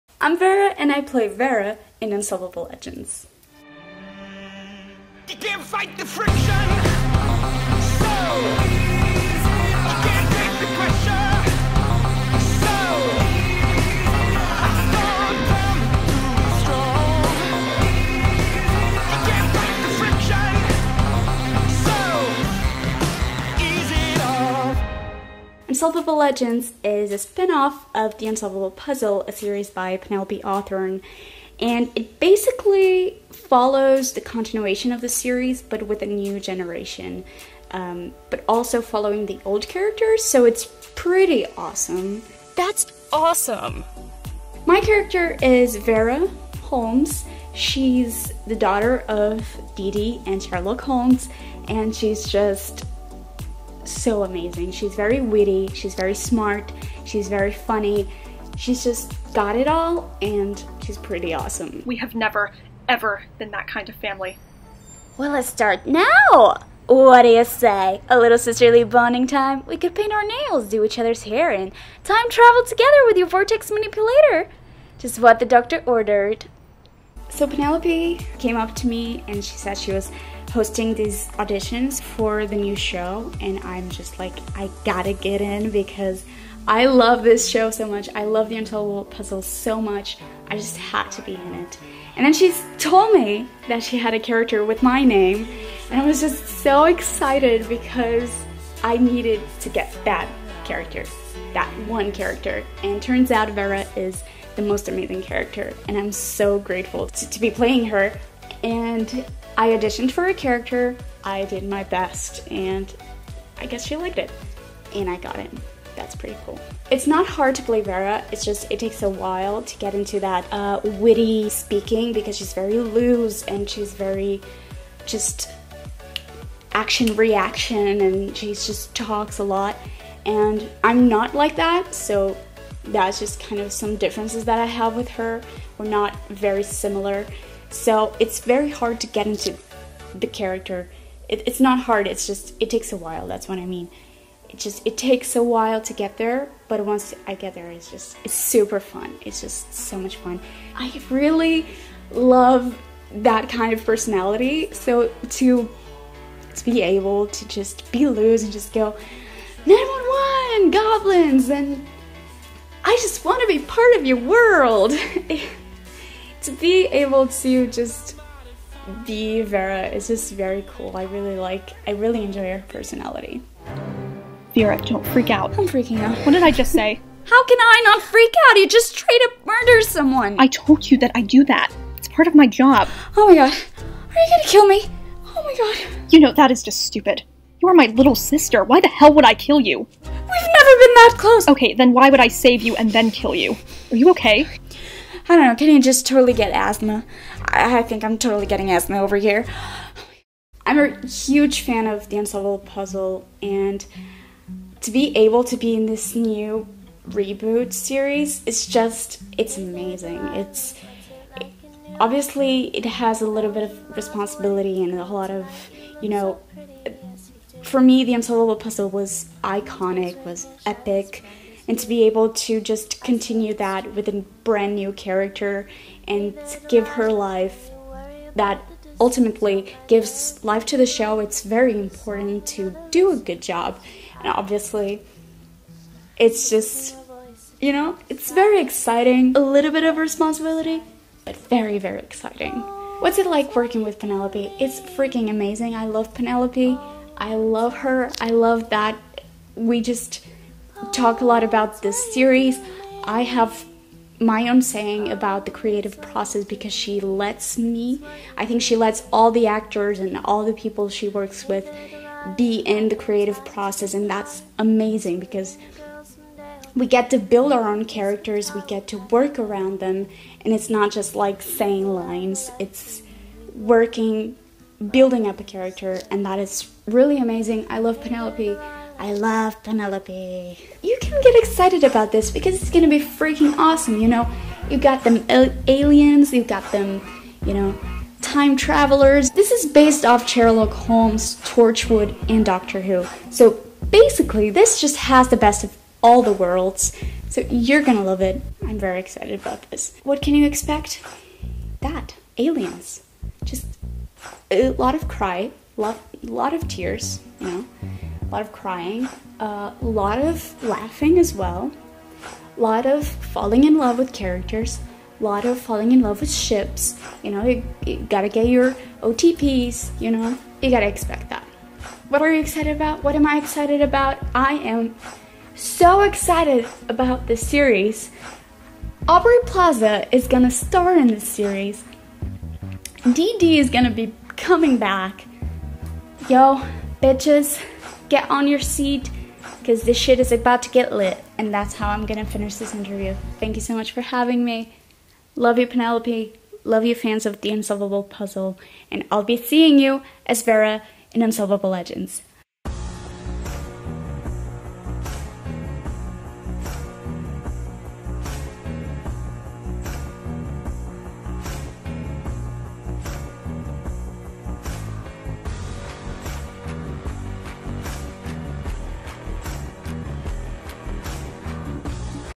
First Look | Legends: Interview with Vera (+Exclusive Sneak Peaks) | PH
Meet Vera [xxbakingcookiesxx] who plays "Vera" in Unsolvable Legends (coming soon!).